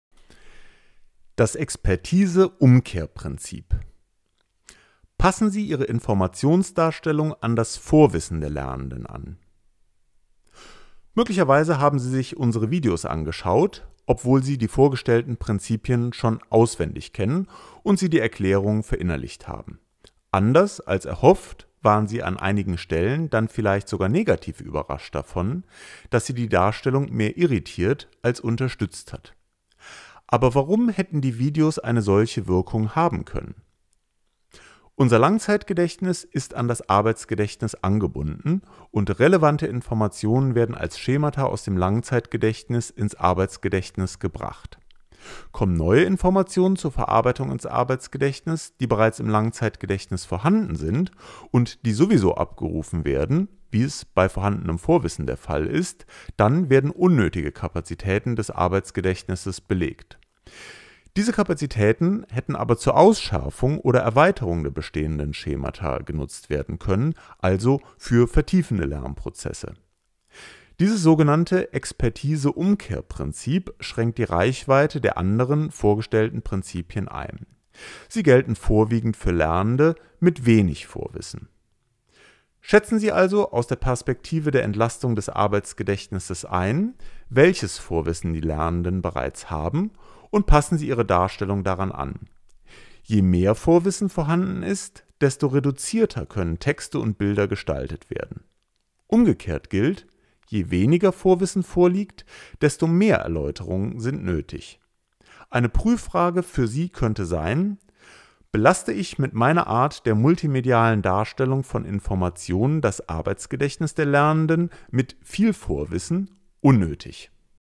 Expertise Umkehr Prinzip
In diesem Video wird das Expertise Umkehr Prinzip auf Basis der kognitive-load Theorie erklärt. Das Video ist Bestandteil des Online-Selbstlernkurses "Erstellung lernförderlicher Arbeitsmaterialien nach der kognitive-Load Theorie". Weitere Informationen finden sie im Fachbereich 35 des NLQ Hildesheim

Das Video wird unter der Lizenz CC-BY 4.0 veröffentlicht. Für die Namensnennung soll genannt werden: Niedersächsisches Landesinstitut für schulische Qualitätsentwicklung (NLQ)